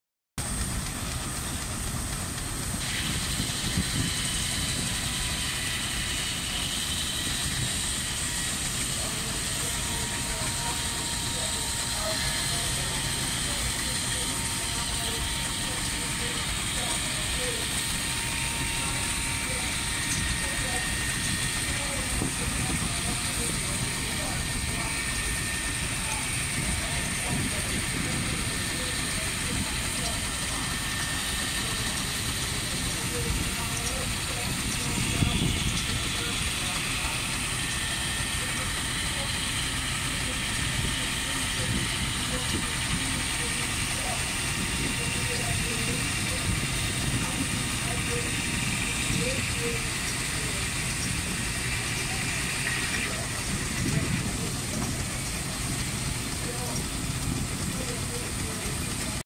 Steam sawing at Great Dorset steam fair
Steam traction engine Steam Sawing. Enjoy!